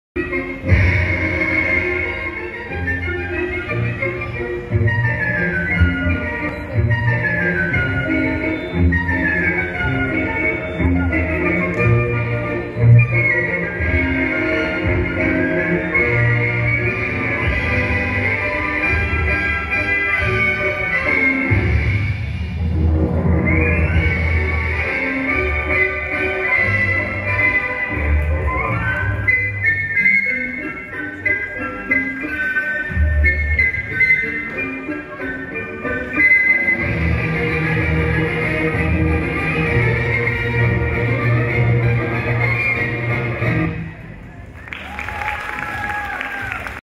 LUCIA OLIVERA EST RÍTMICA PREINF ARO
Torneo Nacional de Clubes de Gimnasia Rítmica
Sede Country Club Estudiantes de la Plata